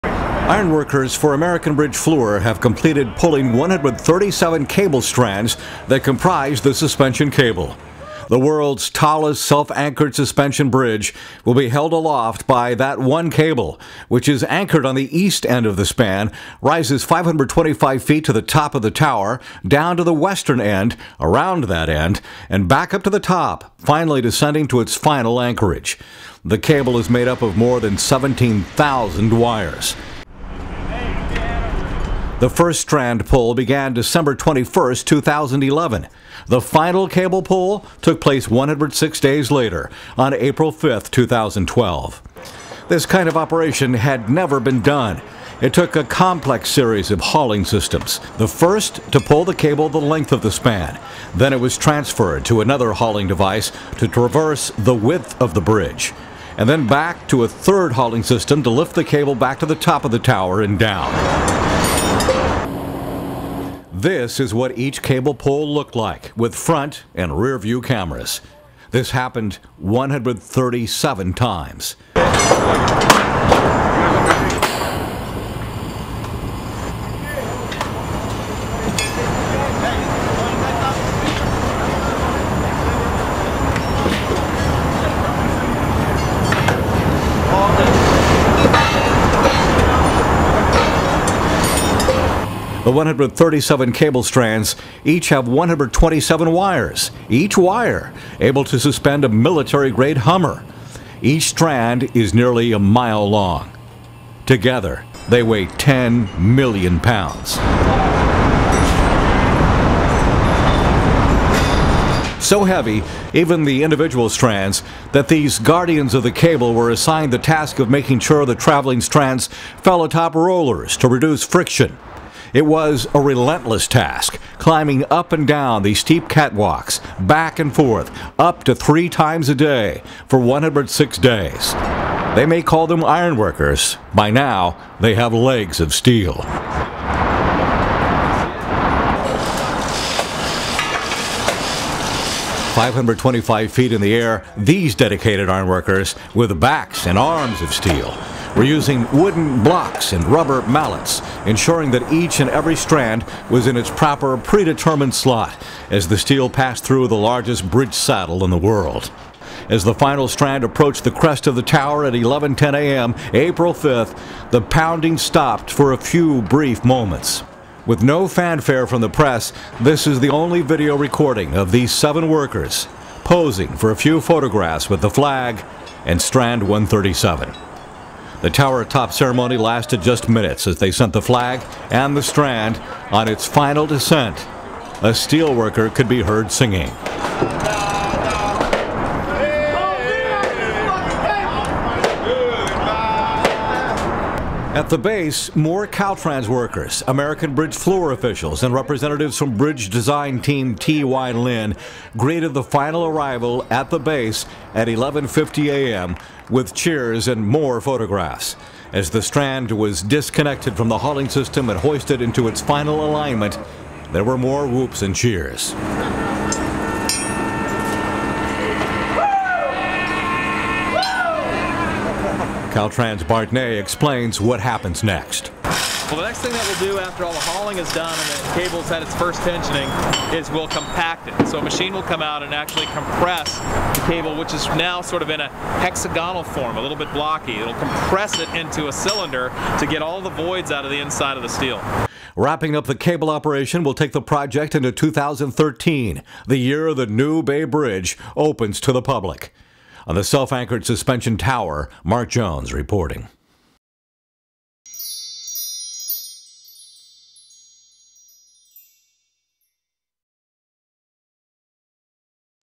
New Bay Bridge Cable Pull Completion
The New Bay Bridge has it's suspension cable!
It has been a one-of-a-kind operation that just ended, a month early.